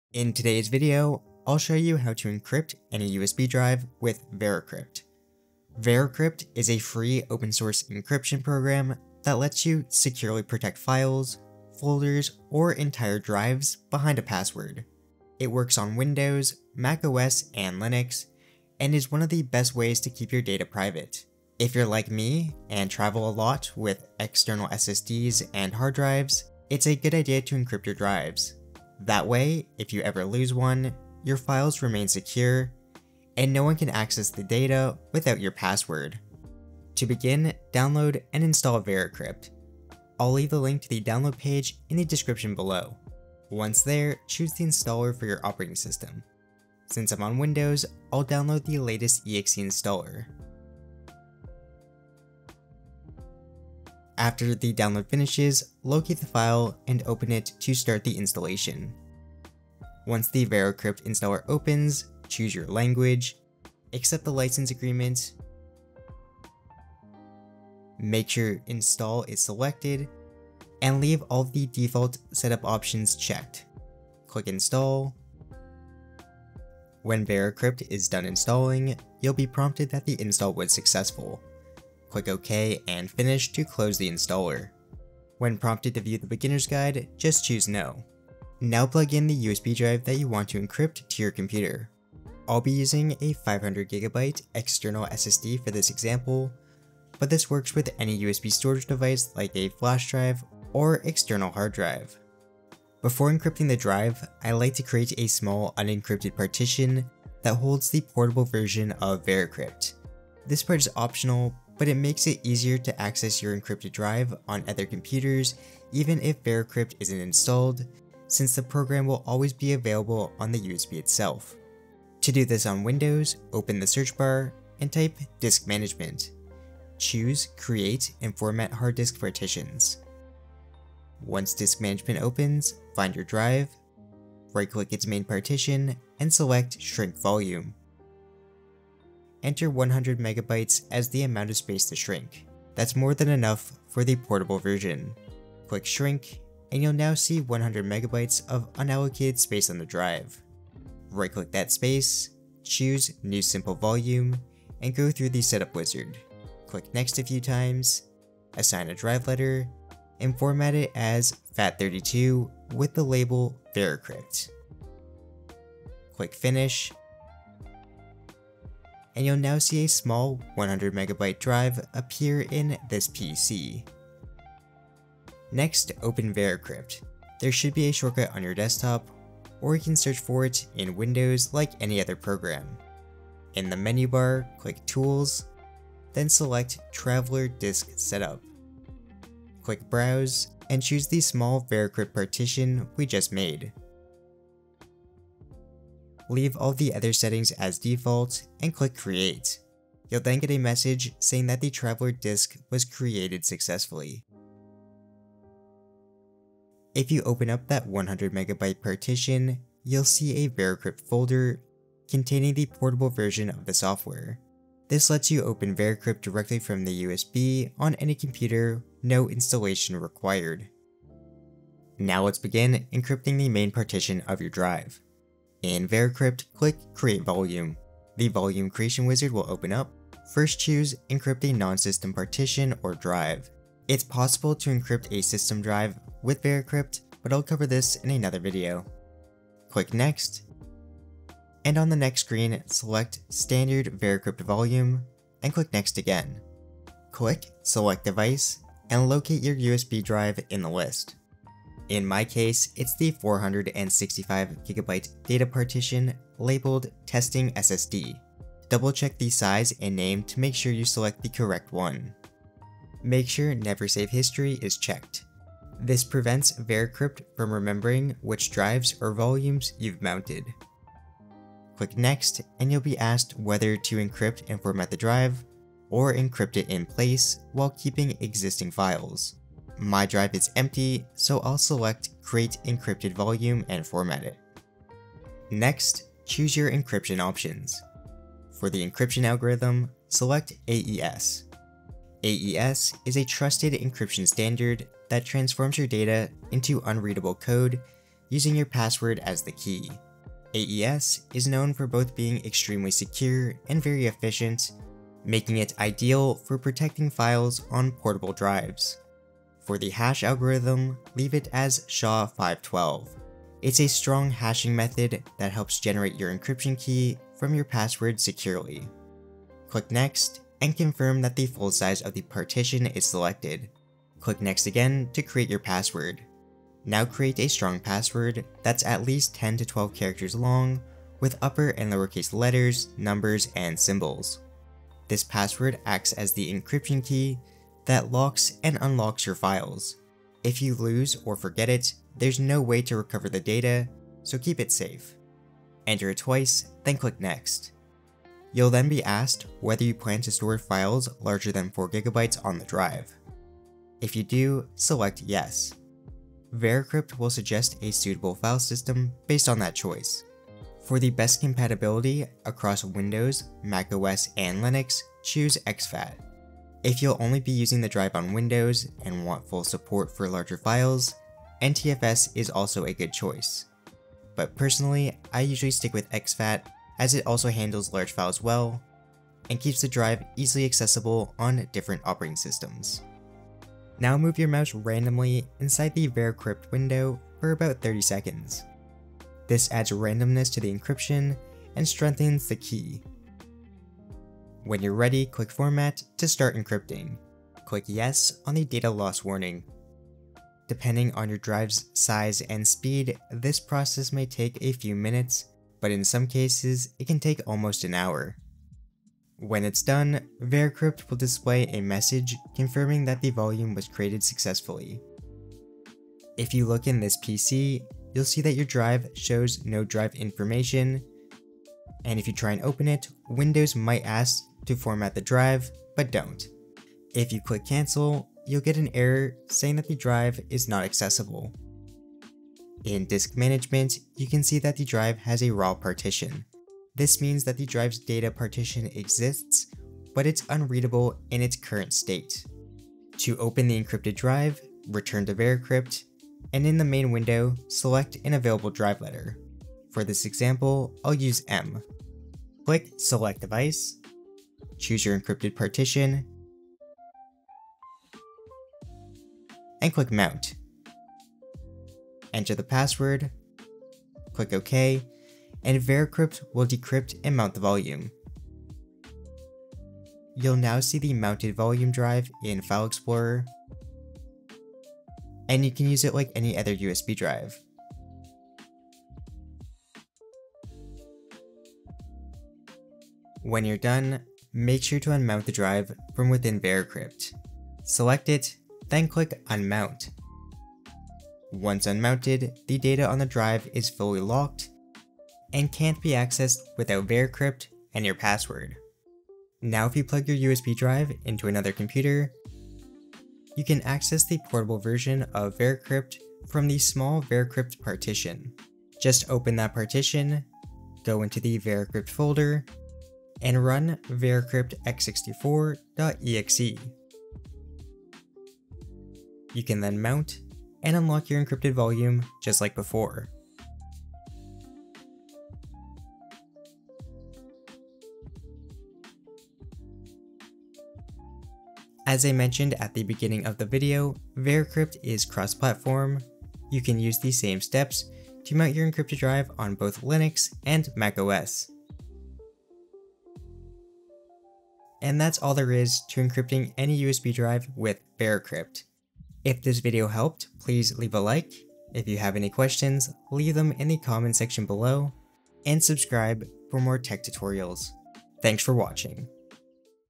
How To Encrypt Any USB Drive With VeraCrypt
🗒️This video will show you how to encrypt any USB drive using VeraCrypt, a free and open-source encryption tool that keeps your files safe and password-protected. In this tutorial, I’ll walk you through the full setup — from installing VeraCrypt to creating, mounting, and using your encrypted drive.

☕This video help you out? How about buying me a coffee

📌 Timestamps:
0:00 - Intro
0:36 - Download & Install VeraCrypt
1:42 - Creating a VeraCrypt Traveler Disk (Optional / Windows Only)
3:42 - Creating Your Encrypted Volume
7:23 - Mounting Your Encrypted Drive
8:19 - Mounting Your Encrypted Drive On Another Computer
8:51 - Mounting Your Encrypted Drive On Linux
9:04 - Outro

📽️Stock Footage:
Glowing padlock appearing